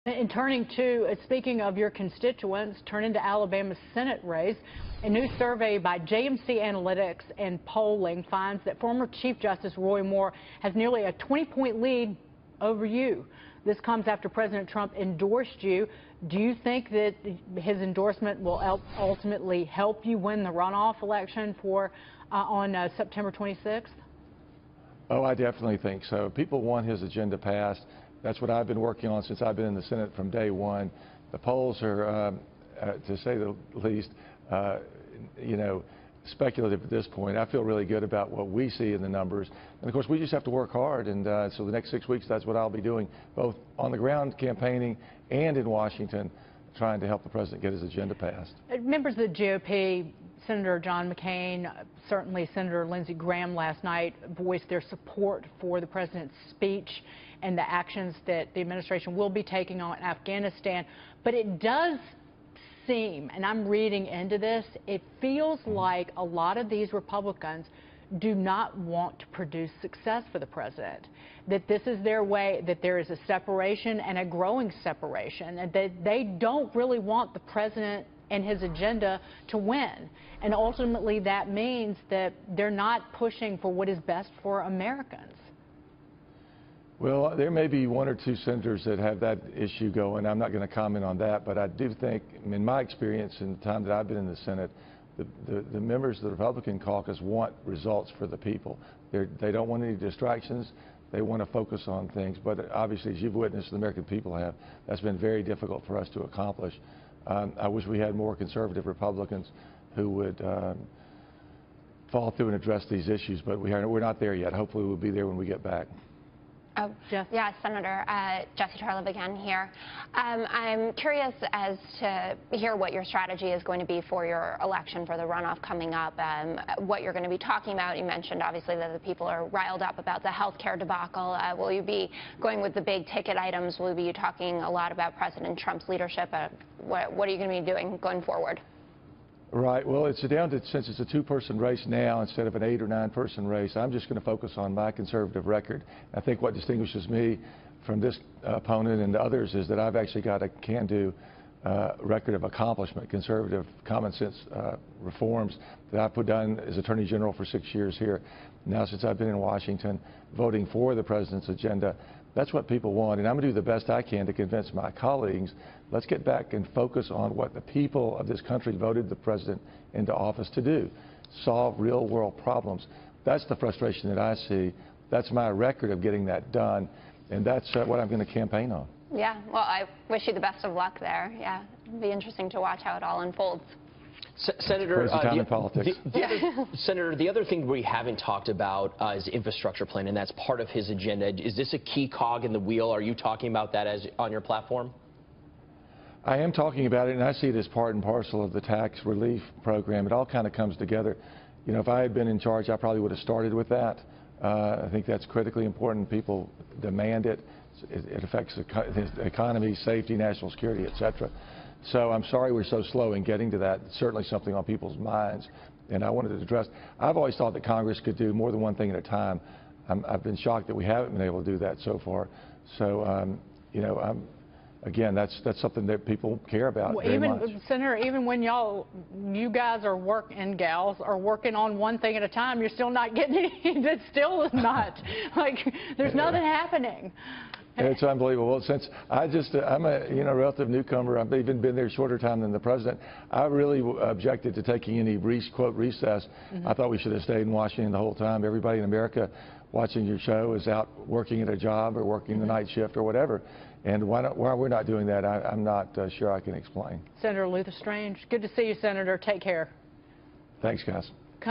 Will Trump’s endorsement help Sen. Strange in Alabama Senate race?
Sen. Luther Strange (R-Ala.) on his Senate race in Alabama and President Trump's agenda.